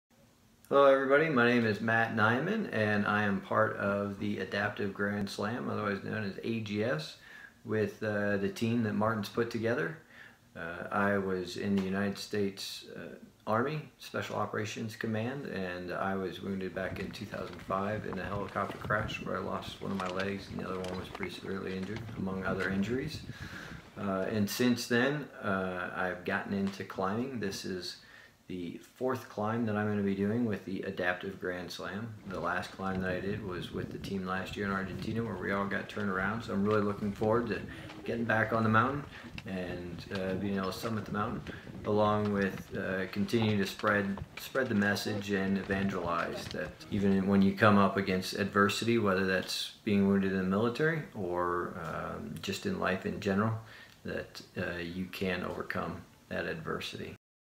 Meet The Team: Matt Nyman
Matt Nyman​ has successfully summited Mount Denali, Mount Elbrus and Mount Kilimanjaro. He was also a member of the Adaptive Grand Slam team that attempted Mount Aconcagua in December 2015, so he knows what to expect in January.

For Matt, the AGS is broader reaching than the mountains the team climb, for him adversity equals opportunity.